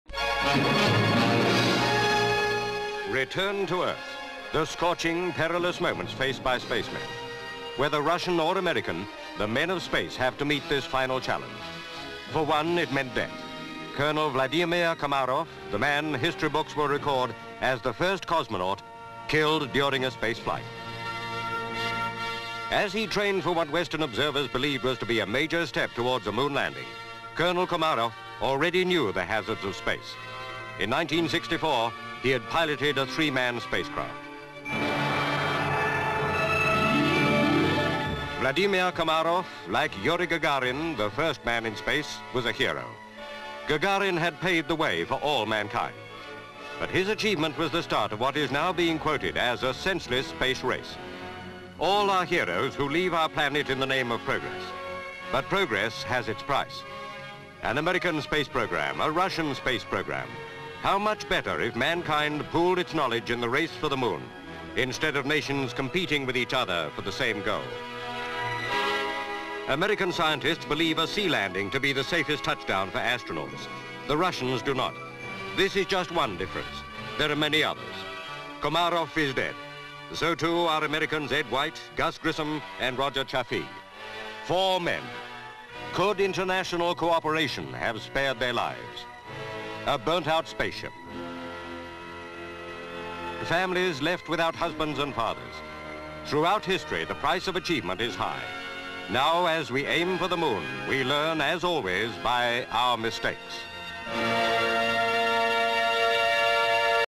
Death Of Russian Spaceman (1967)
No title - Death of Russian spaceman Colonel Vladimir Komarov.

Various shots of a spaceship on re-entry into Earth's atmosphere. C/U and M/S newspaper headlines announcing the death of Komarov.  C/U Komarov's picture. C/U headlines 'Spaceship falls 4 1/2 miles in landing failure'.  Various shots of Komarov in training floating about inside capsule.  C/U inside the first space capsule for three men which Komarov piloted in 1964. C/U Komarov.

M/S of Yuri Gagarin.  M/S of crowd. M/S Gagarin with Nikita Khruschev. M/S of Ed White doing American walk in space. Various shots of space capsule in flight.  M/S of a sea landing.  C/U's of the three American astronauts, Ed White, Virgil Grissom and Roger Chaffee, who were killed by a fire in their capsule.   Various shots of the burnt-out American space capsule. M/S of Komarov with his family. C/U Komarov.  M/S of the moon.
 FILM ID:2027.22